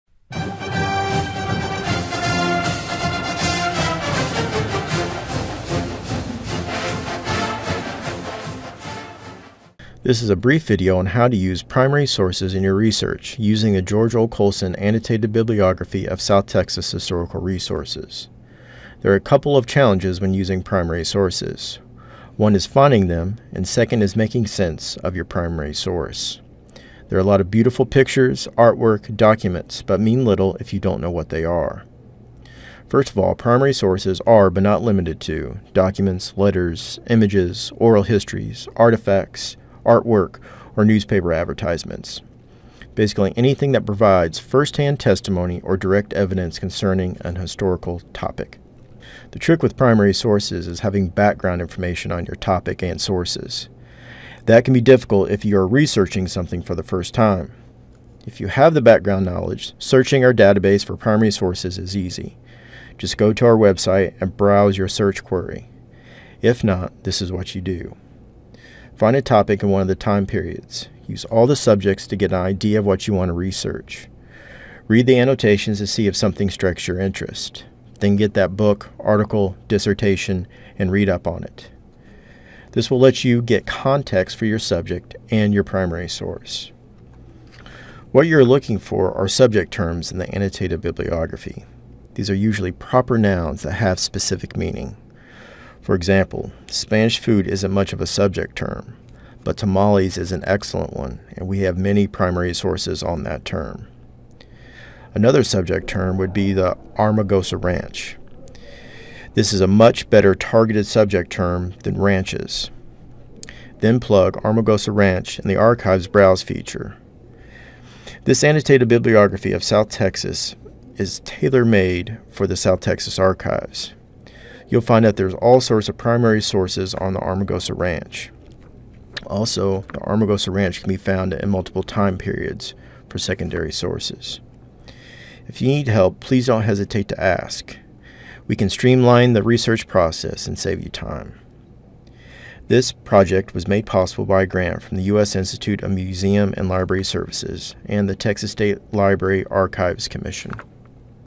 How to look for primary sources at the South Texas Archives of Texas A&M University-Kingsville.
How to look for primary sources in the South Texas Archives and using the George O. Coalson Annotated Bibliography of South Texas Historical Resources to find subject terms and research topics.

This project is made possible by a grant from the U.S. Institute of Museum and Library Services and Texas State Library and Archives Commission. (2019, 2020) (Grant Numbers TXT-19006 and TXT-20009).

The link to the George O. Coalson's Annotated Bibliography of South Texas Historical Resources
The link to the U.S. Institute of Museum and Library Services
The link to the Library Services and Texas State Library and Archives Commission